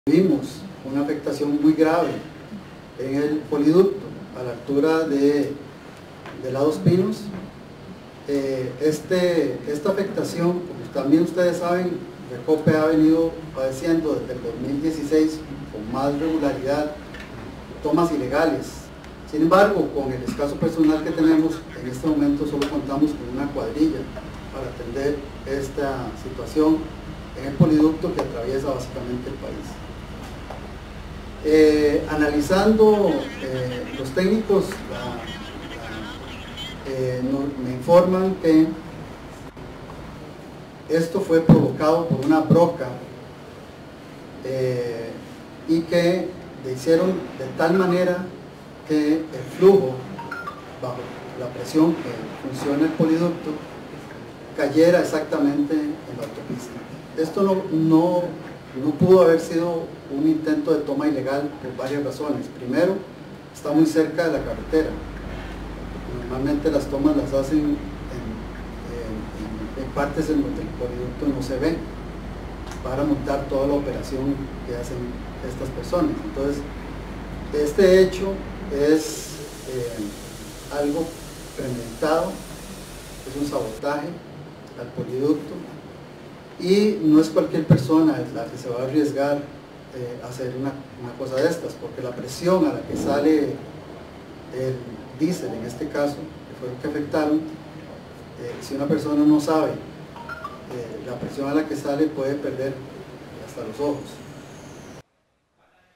Octavo día de huelga de sindicatos en Costa Rica
Costa Rica inició el octavo día de huelga con un atentado contra la tubería de petróleo que atraviesa el país. El detalle de este incidente lo conocemos del Presidente  Ejecutivo de la Refinadora Costarricense de Petróleo; RECOPE, Alejandro Muñoz.

Redacción CB24

24 Horas conectados con la información